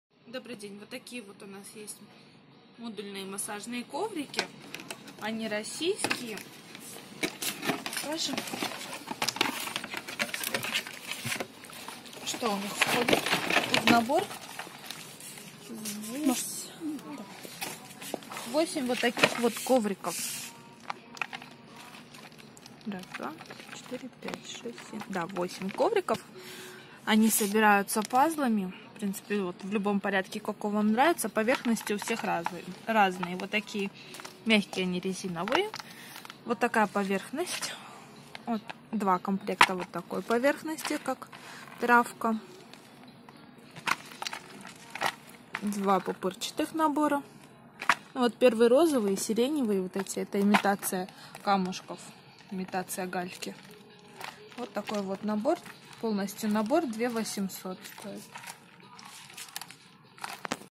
Массажный коврик Орто обзор от 17012019
Массажный коврик Орто обзор от 17012019 Посмотреть и купить можно в магазине Крошка-Бишкек